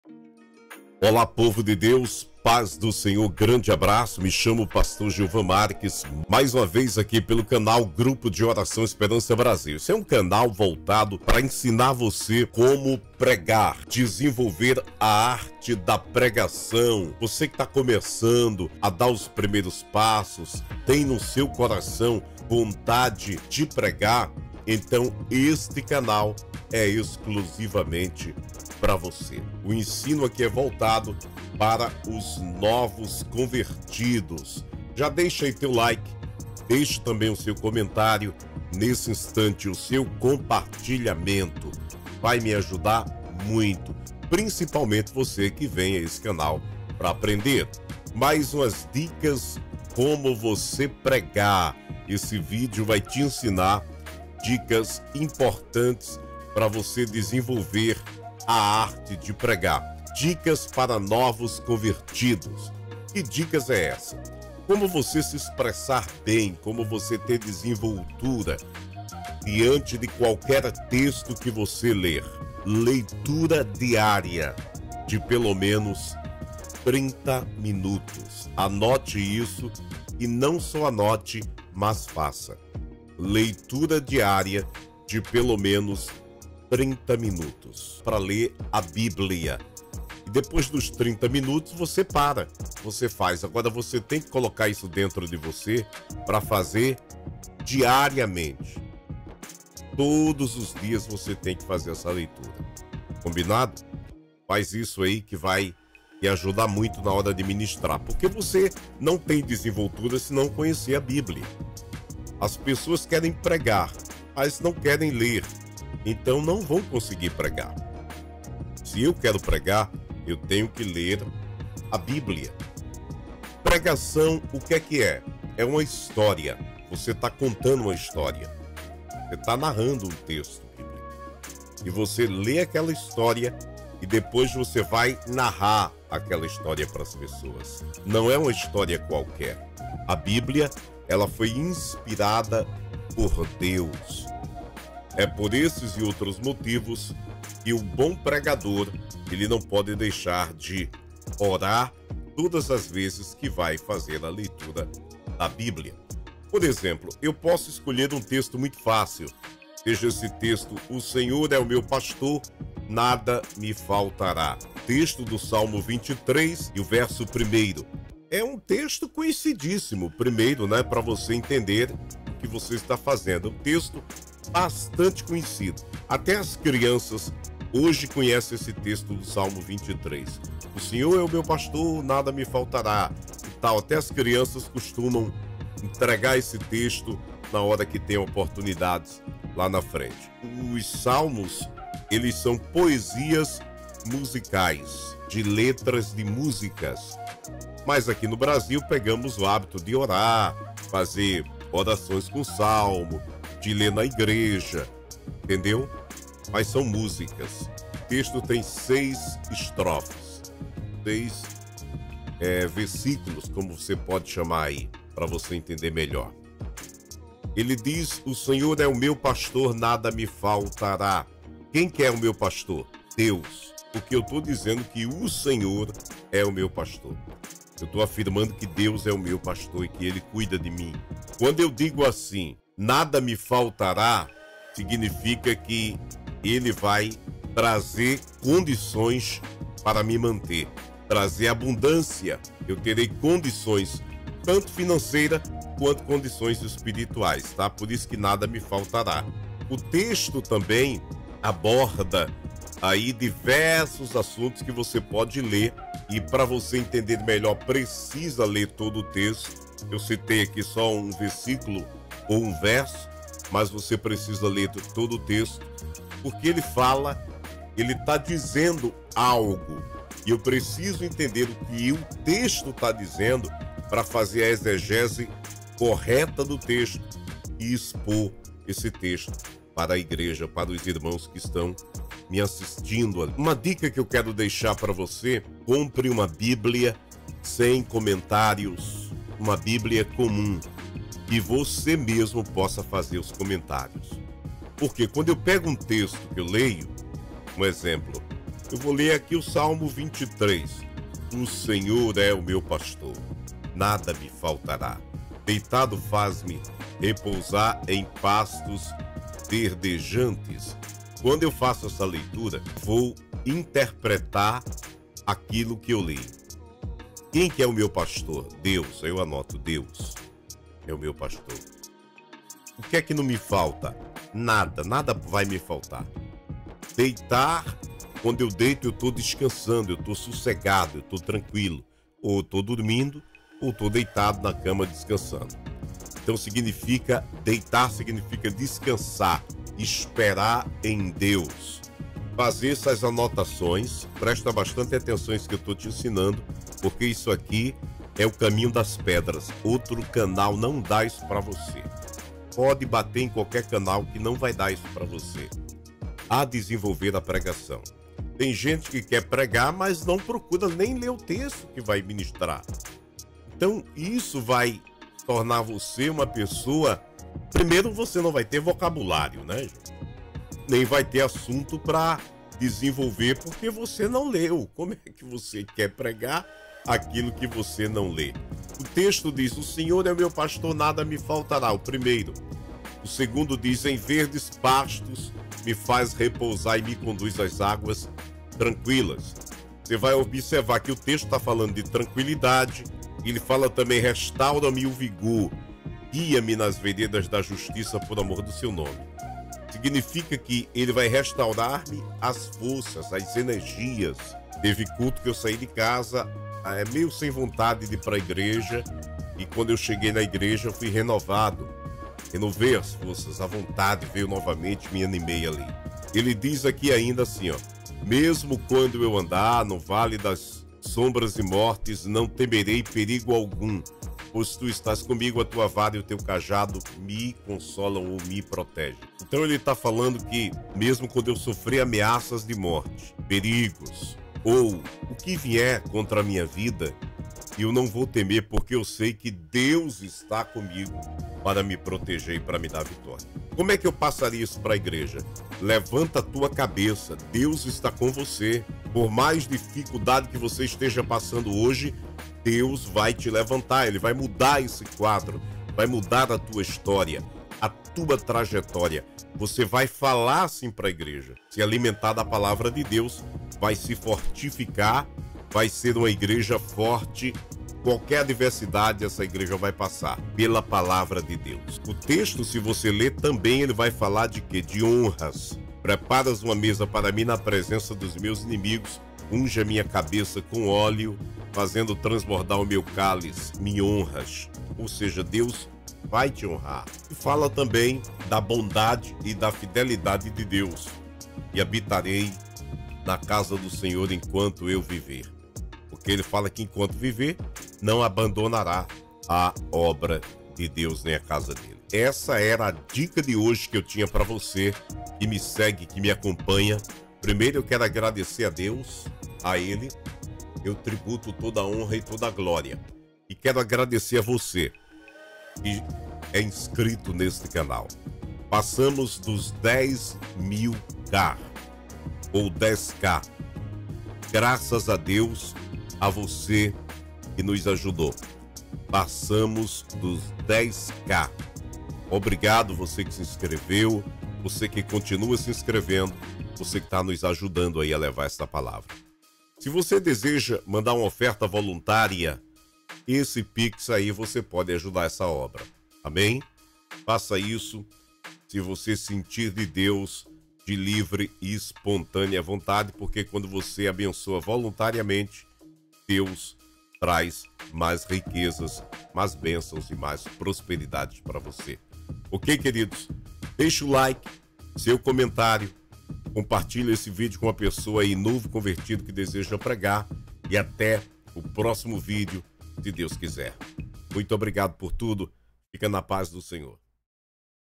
COMO PREGAR -Aprenda fazendo o que ensino neste vídeo 🙏✍🎤🚀📘
▶ Neste  video trago ensinamento como voce pregar direto ao ponto. assiste este vídeo agora !!!